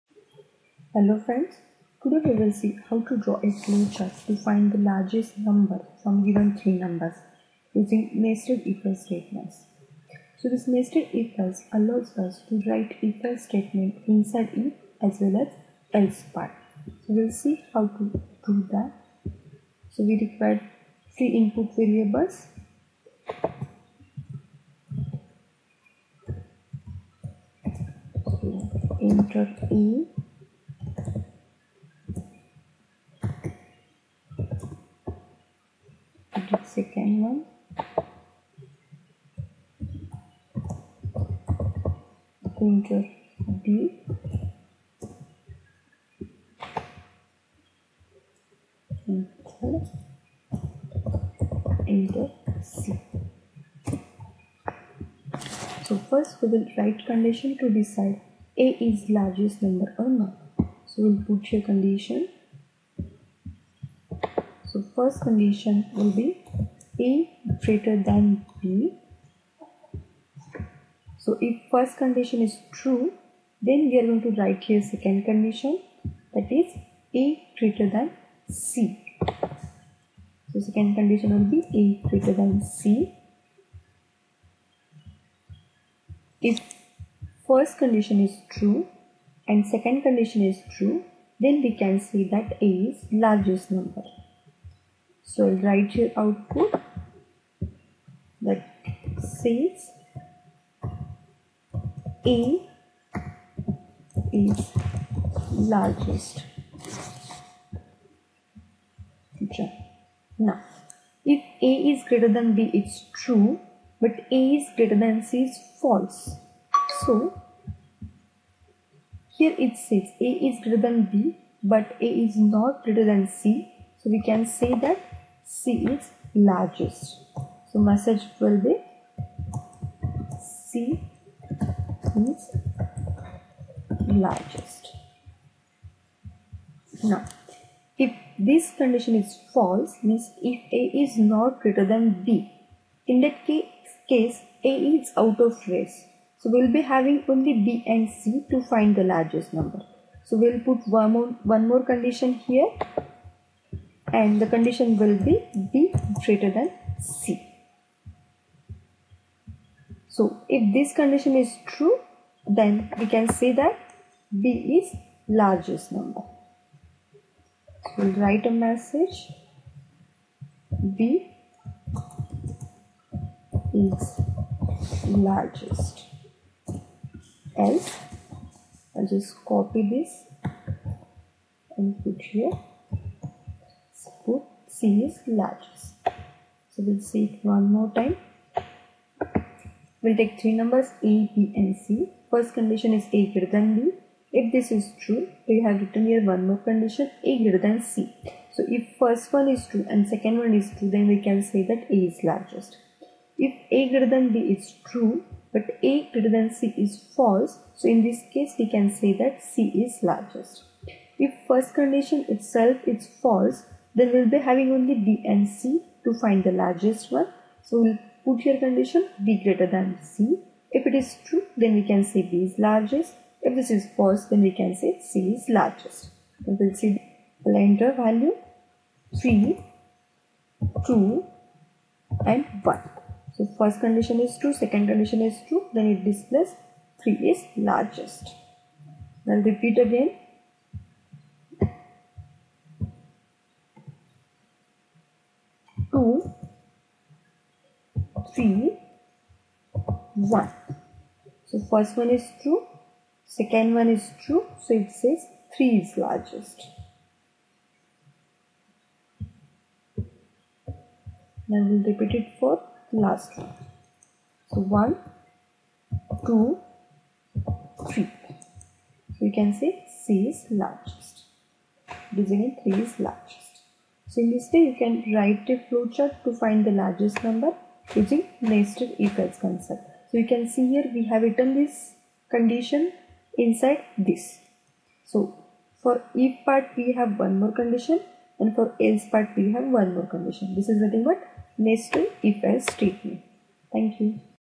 RAPTOR: Flowchart to find Largest number among 3 using Nested If Else
This video explains the flowchart for how to find the largest number from given 3 numbers. for this Nested If Else statement is used.